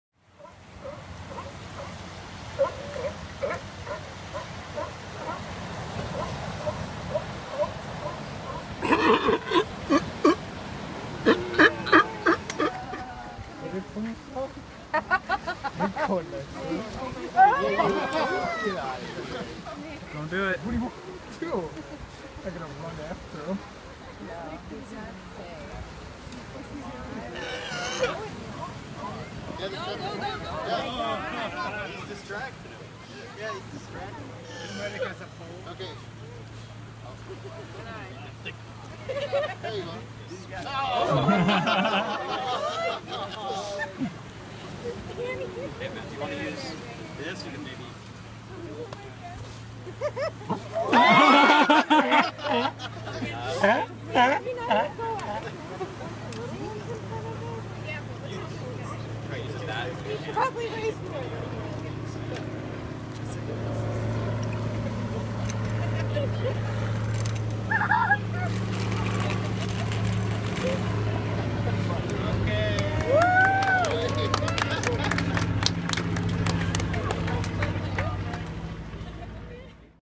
Sea Lion claims tourist's shades - La Jolla, California - 2014
A tourist attempts to recover his sunglasses after dropping them in front of a proud and territorial Sea Lion.